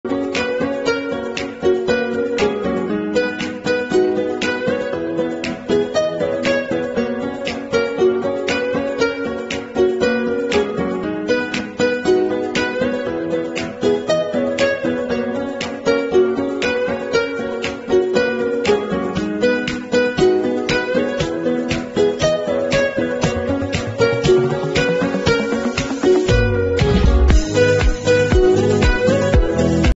Coffee printer,instead complicated latte art, simple operate and fun!
The coffee printer can upload any pictures you want from your phone , print your pictures on coffee,beer,bread and cookies! Can help you cut the cost of latte artist, enrich the fun of beverages!